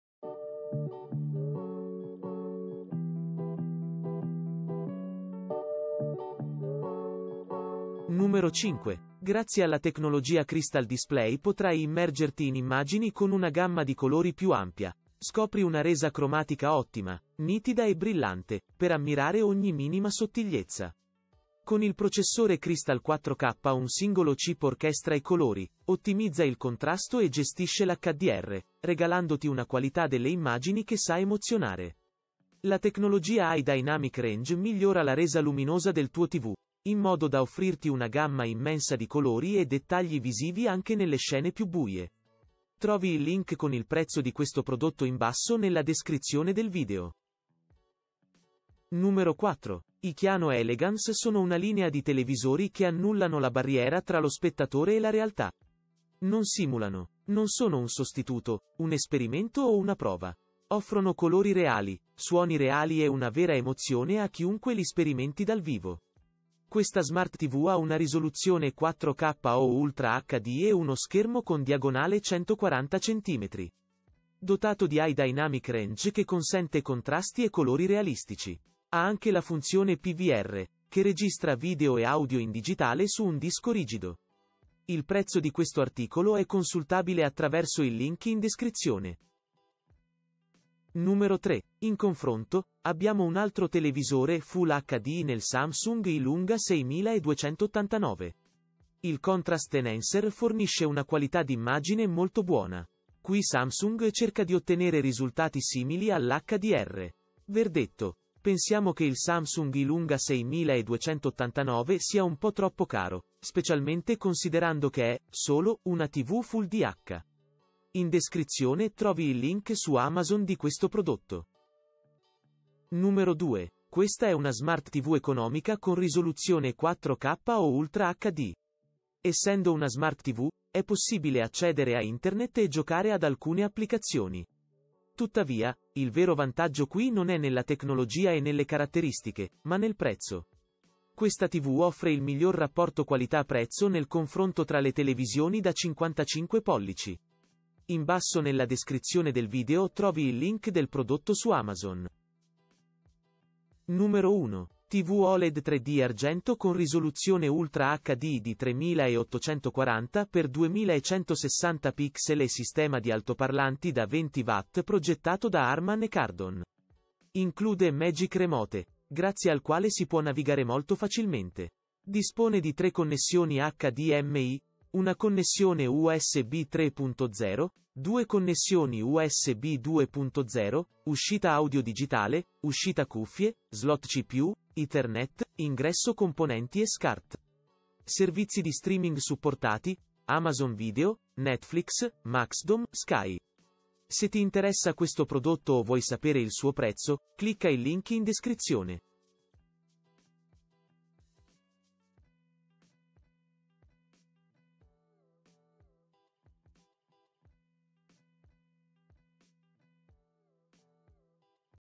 i 5 migliori televisori Smart TV da 55 pollici
1) LG 55EF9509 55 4K Ultra H... »
2) LG 55UH605V TV LED 55 4K ... »
3) Samsung Tv - Samsung UE40... »
4) Kiano Elegance TV 55 Poll... »
5) Samsung TV UE55TU7190UXZT... »

✋ Ecco i 5 migliori televisori smart tv da 55 pollici, disponibili sul mercato.

00:00 - Intro
00:08 - 5) Samsung TV UE55TU7190UXZT Smart TV 55” Serie TU7190, Crystal...
00:48 - 4) Kiano Elegance TV 55” Pollici 4K UHD HDR10 (Android TV 9.0 2...
01:30 - 3) Samsung Tv - Samsung UE40J6289, televisore (ultra HD, triple...
02:02 - 2) LG 55UH605V TV LED 55” 4K Ultra Hd Smart TV WiFi EUROPA Grey...
02:33 - 1) LG 55EF9509 55” 4K Ultra HD Compatibilità 3D Smart TV Wi-Fi ...